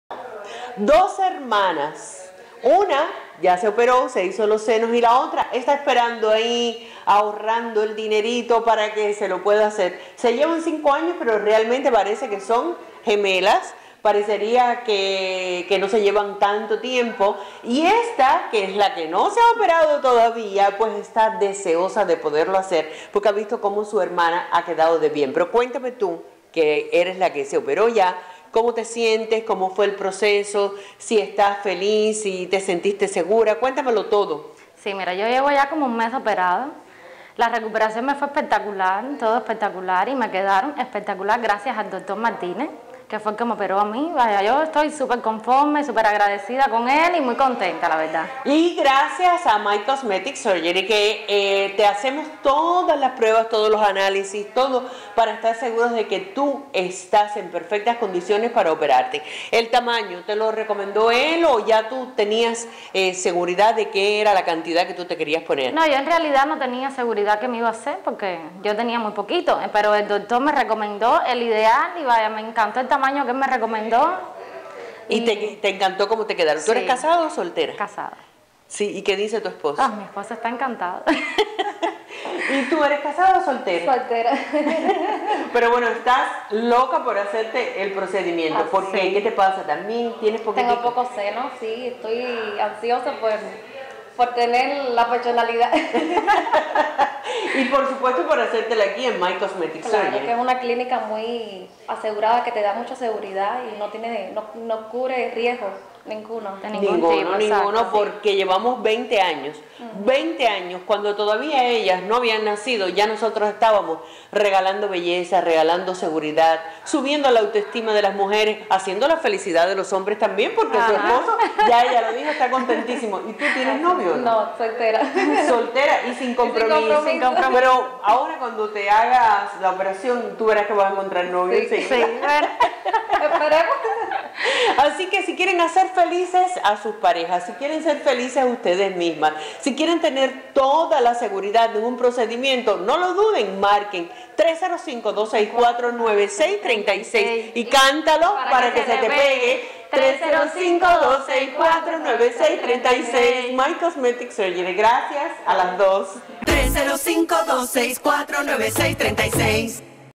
HERMANAS EN MY COSMETIC SURGERY
Conoce la historia de estas dos hermanas: Marbelis y Mairely Duarte. Una se acaba de operar en My Cosmetic Surgery, y la otra está esperando por el momento de hacer realidad su sueño. Conoce el resto de la historia en este video.